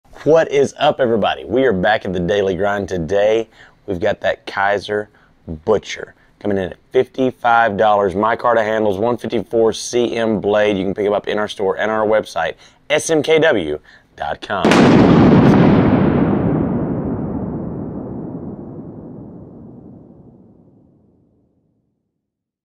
KIZER BUTCHER
No matter if you add it to your EDC or just use it in camp/kitchen the Eddie Diaz designed Kizer Butcher is awesome! With a 2.5" 154CM blade and 5.75" over all it is big enough to tackle any job but not so large it gets in the way of carrying it. Kydex belt sheath included for horizontal carry.